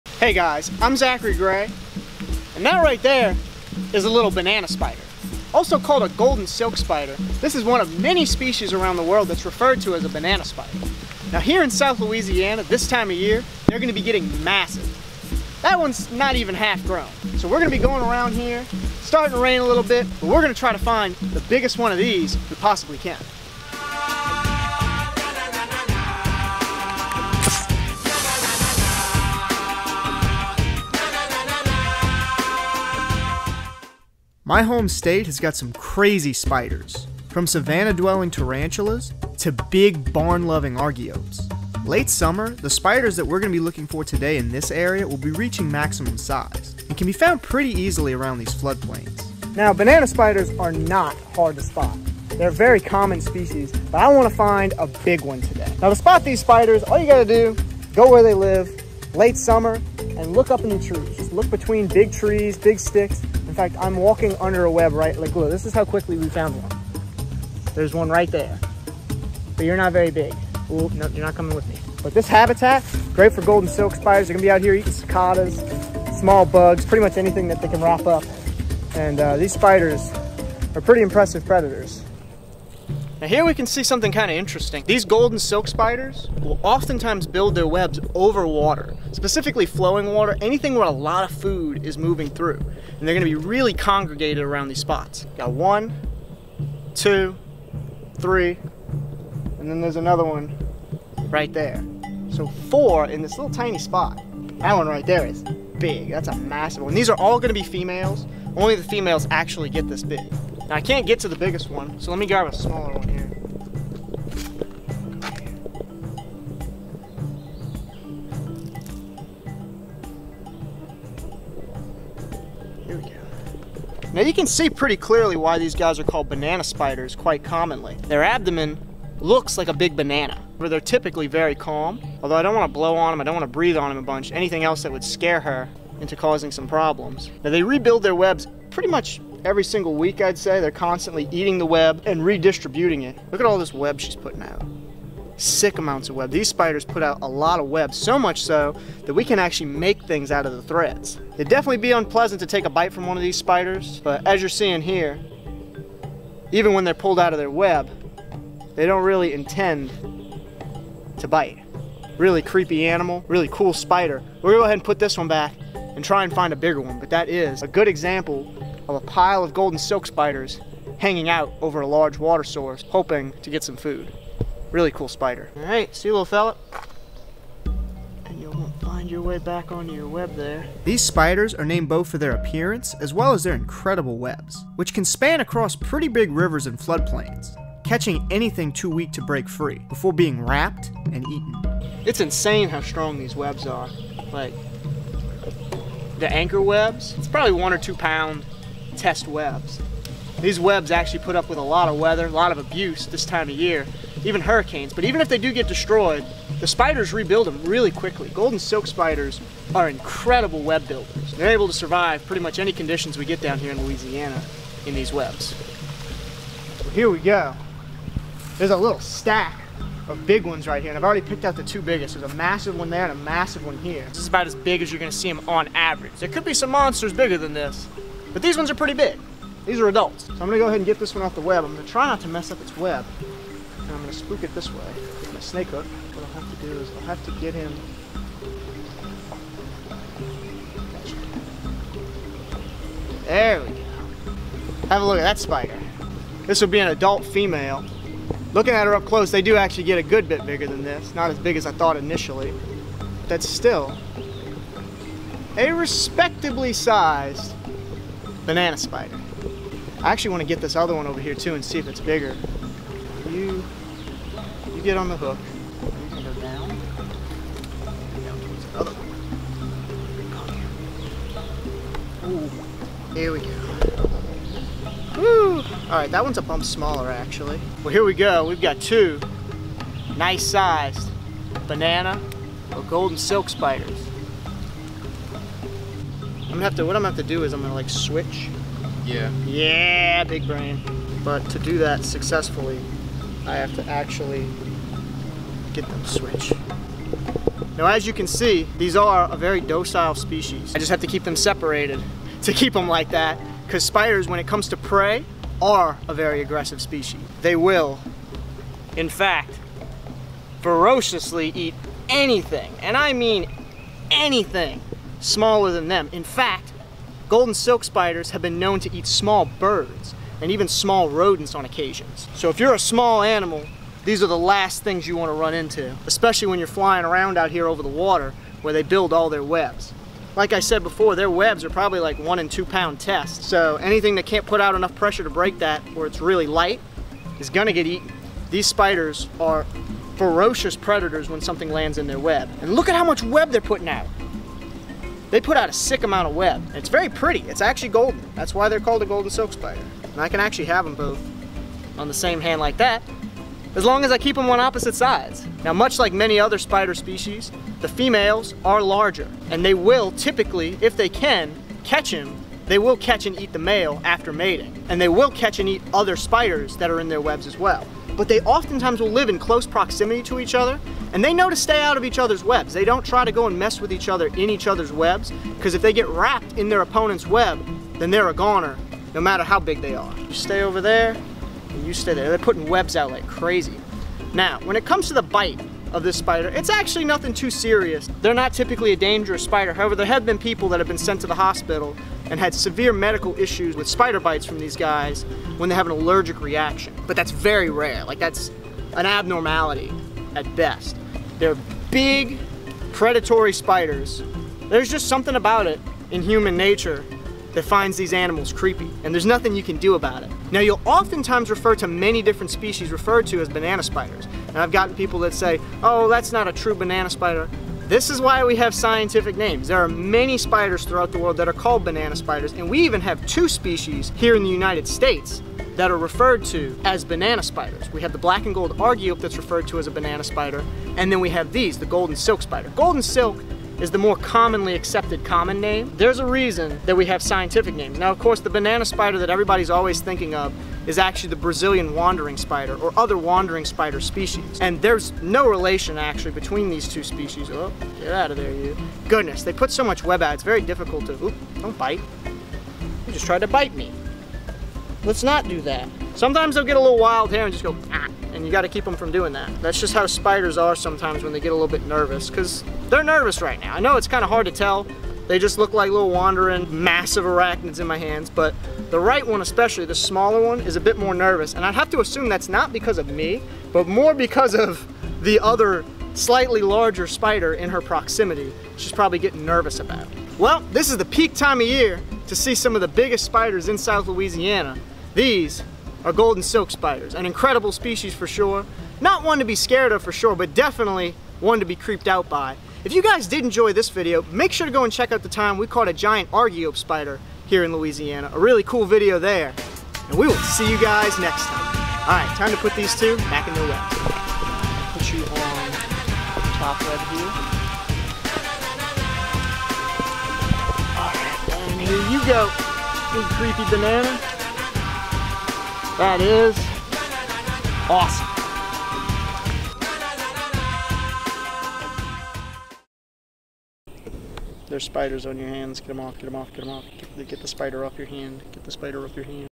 American Banana Spiders! Are they ACTUAL Banana Spiders?.....
On today's adventure, we're out to catch some spiders! And not just any spider. one of the largest and most widely known species in the southern United States which happens to share a nickname with one of the worlds most deadly spiders: the banana spider. BUT are these giant spiders ACTUALLY banana spiders?. Well. KIND OF! The golden silk spider's banana shaped and colored body earned itself the nickname, and due to this commonly shared nickname with the brazillain wandering spider, many people fear this animal, so our goal is gonna be to get up close with some of the biggest banana spiders we possible can, and find out just what these spiders are really like! As always we hope y'all enjoy this video :D

Wanna keep up to date with all our adventures?